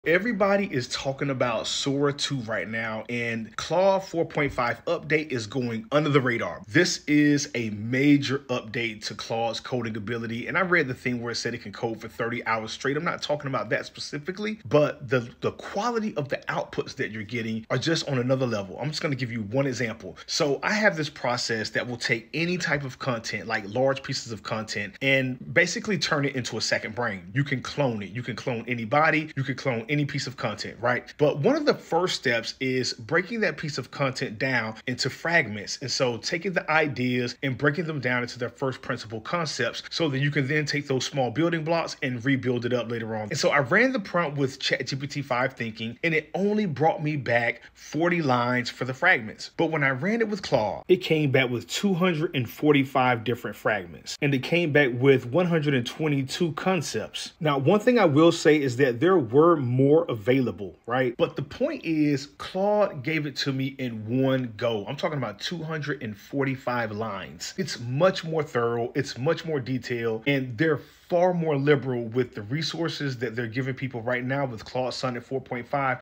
Claude Sonnet 4.5 Is Insane!
Everyone is talking about Sora 2.
Meanwhile, the Claude 4.5 update is flying under the radar.
This release meaningfully boosts coding and long-context reasoning.
I’m not focused on the “30-hour coding” claim.
I’m focused on output quality.

Here’s one concrete test from my workflow.
I run a “Second Brain” pipeline that breaks any long piece of content into atomic fragments and first-principle concepts.
Same source. Same instructions. Different models.

* GPT-5 Thinking returned **40** fragment lines.
* Claude 4.5 returned **245** fragment lines and **122** concepts—in a single pass.
* There were still “more available,” but Claude delivered far deeper coverage in one go.

Why this matters:
Better fragmentation → higher coverage → cleaner reconstruction of ideas → stronger downstream assets (outlines, articles, code, courses).
If you build systems that depend on recall, traceability, and synthesis, throughput and faithfulness at the fragment level are decisive.

In this video I show:

* The exact prompt structure I use to extract fragments and concepts.
* Side-by-side results (counts, fidelity, noise).
* How deeper fragmentation improves my rebuild step (turning fragments back into usable frameworks).
* When I still reach for other models—and why.

Caveats:

* This is one test on one pipeline.
* Your mileage will vary with prompts, inputs, and constraints.
* I’ll keep testing across books, transcripts, and codebases.

Who should watch:
Developers, researchers, and creators building content intelligence systems, second-brain workflows, or high-coverage RAG.